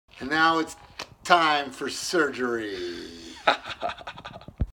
Father and Son iMac surgery
Fred Dewey and Tyler Dewey begin the day-long task of taking apart an iMac to change the graphics card.